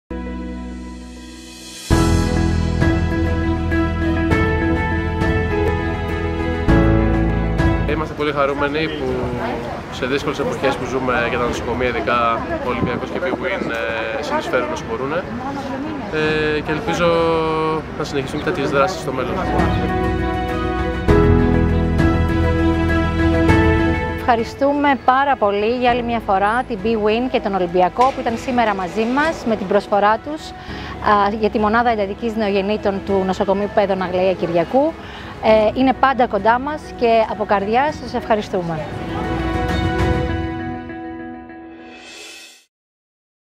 Η bwin και η Κ.Α.Ε. ΟΛΥΜΠΙΑΚΟΣ στηρίζουν το Γ.Ν. Παίδων Αθηνών “Παναγιώτη & Αγλαΐας Κυριακού”
Κάνοντας πράξη τη δέσμευσή τους – στο πλαίσιο του προγράμματος “Mondays Games Against Covid” – για συνολική υποστήριξη του έργου του Νοσοκομείου, η bwin και η Κ.Α.Ε Ολυμπιακός παρέδωσαν δύο σύγχρονες συσκευές φωτοθεραπείας led για τη θεραπεία του νεογνικού ίκτερου στη Μονάδα Εντατικής Νοσηλείας Νεογνών του Νοσοκομείου.